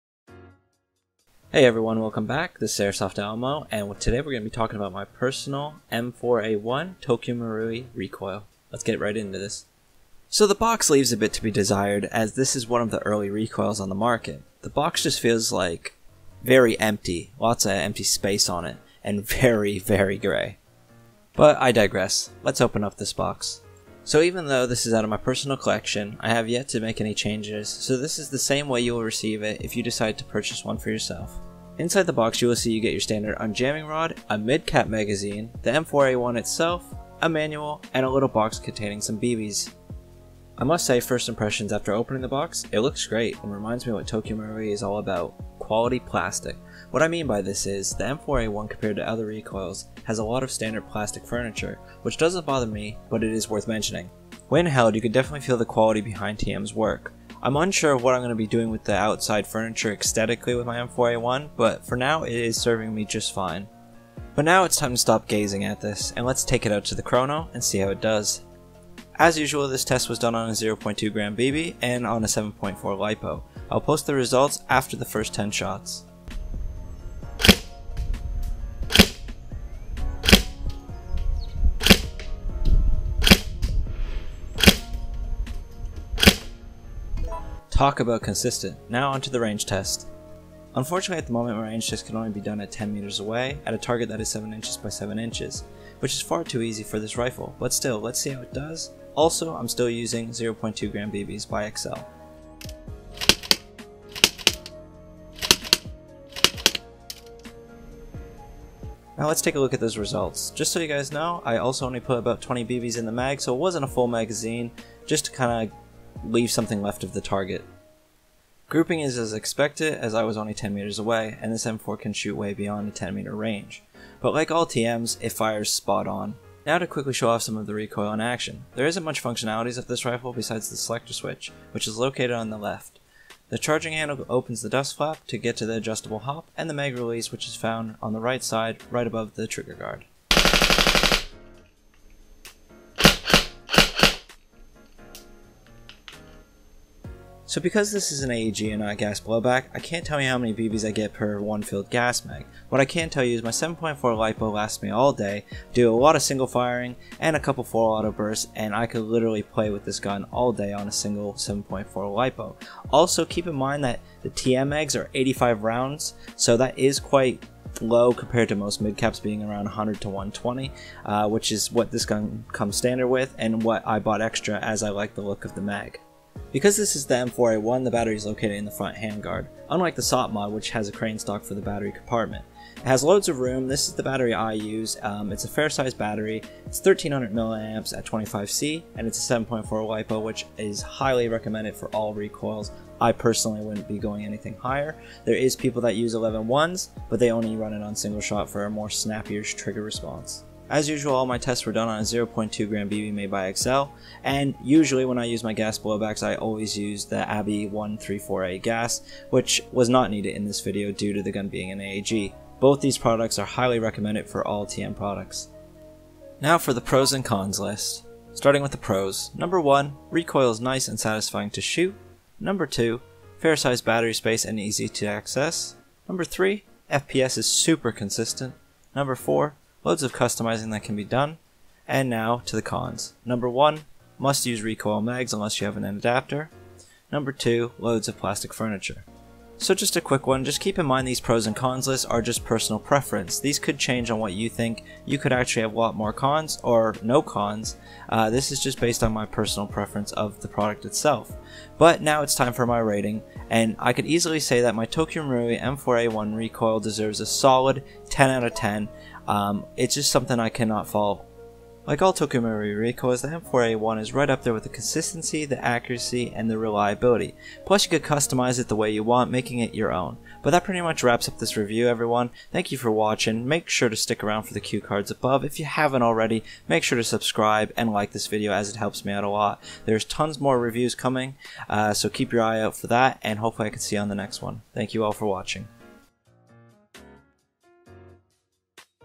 Tokyo Marui M4A1 Recoil Review
From my personal armory my Tokyo Marui M4A1 Recoil!

All tests are done on a 0.20g BB made by Excel